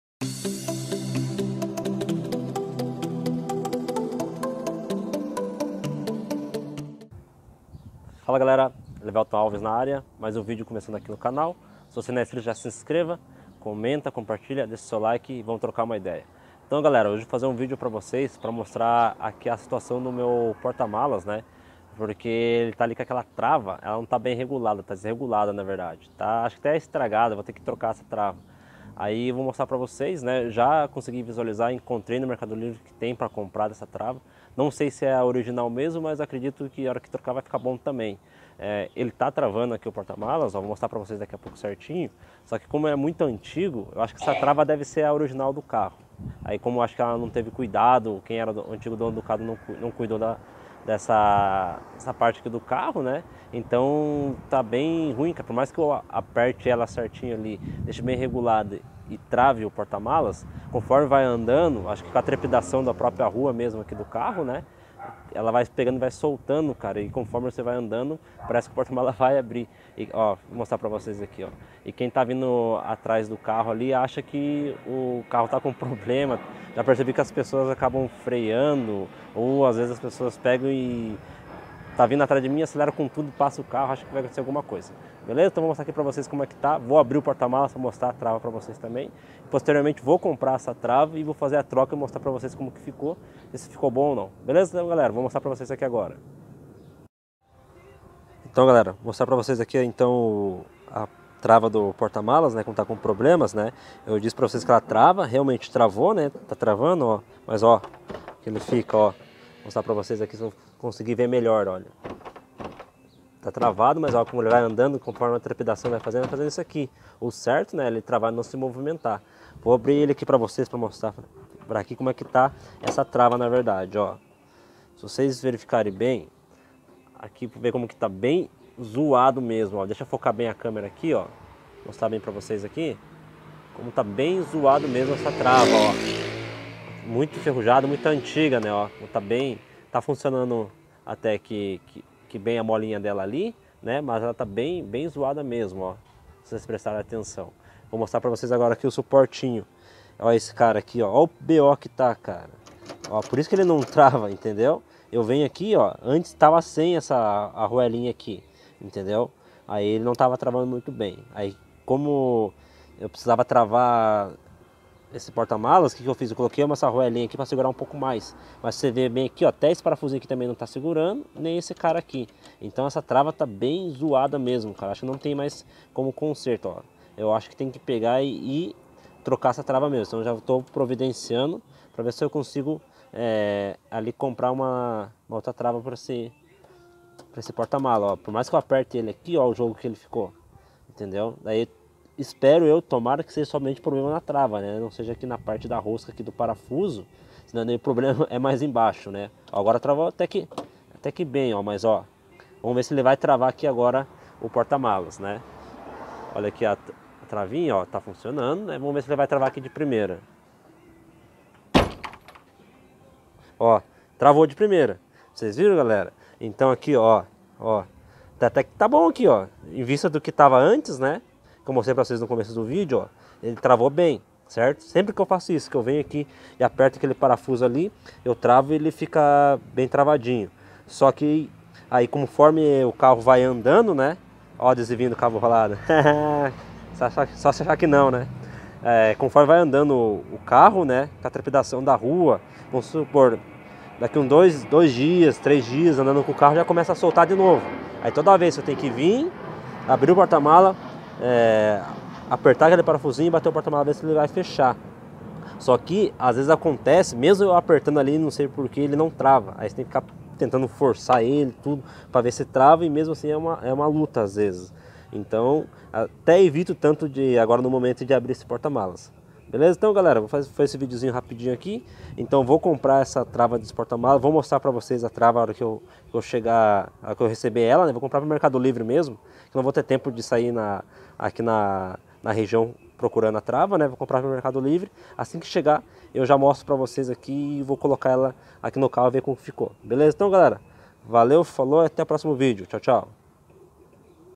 Trava do porta malas do Gol Quadrado com defeito
Fala Galera!!!

Nesse vídeo mostro o problema que está com o porta malas do meu goletinha...

Resolvi o problema galera, segue o video da trava do porta malas consertada

Quer trocar uma ideia?
Manda um salve no insta  @elivelton_alves2012
Noix